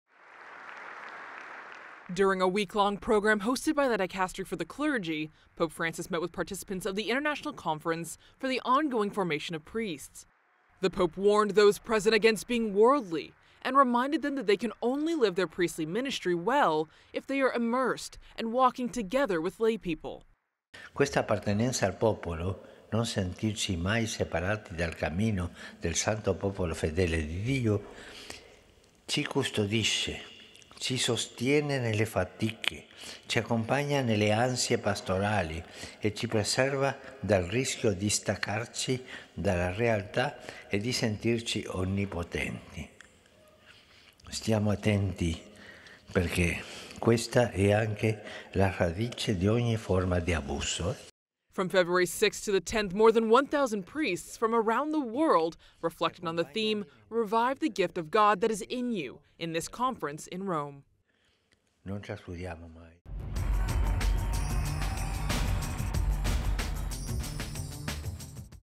VATICAN | Pope Francis warns priests against being worldly as it is “root of all forms of abuse”
The Pope reminded the more than 1,000 priests that they can only live their ministry well if they are “immersed” and “walking together” with lay people.